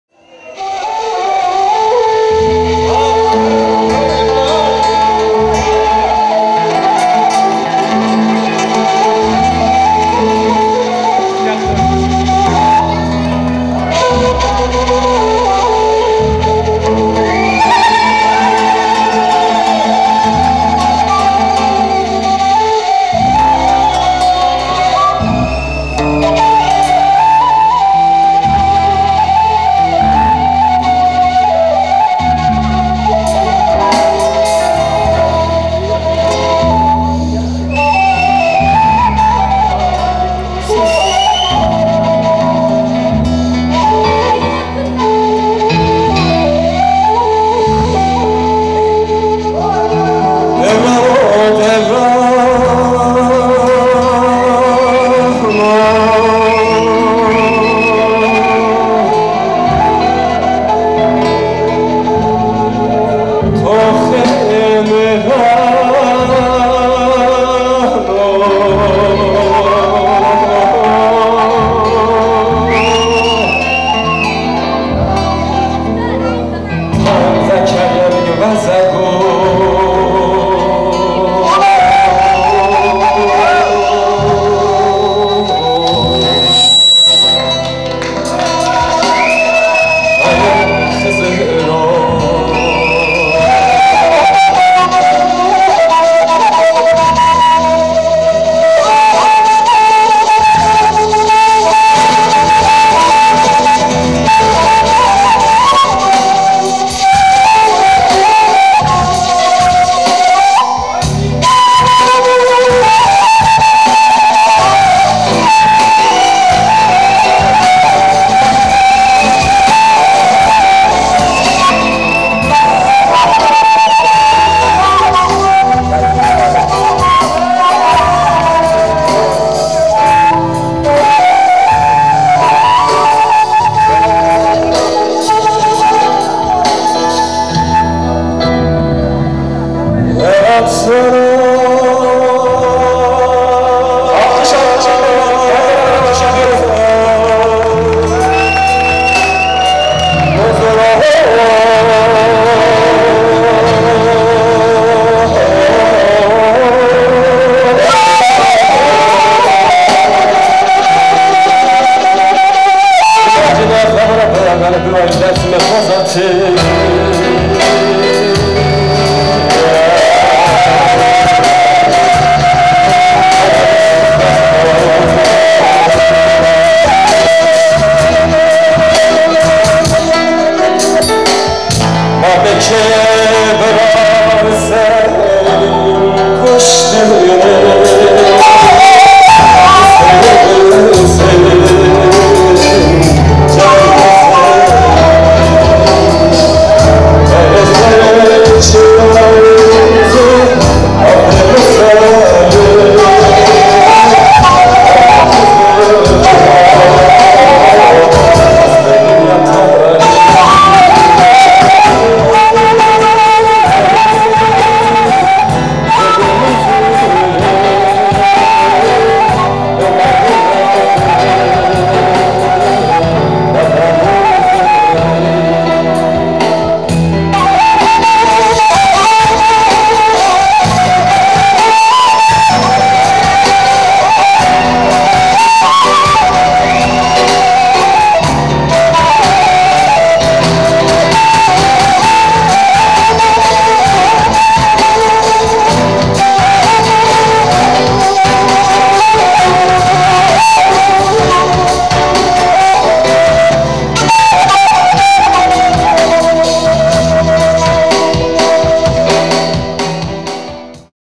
Feryat, Dilo, Guno, Umo kaval guitar bass baterie piano
mariage a gamze feriyat met le feu :)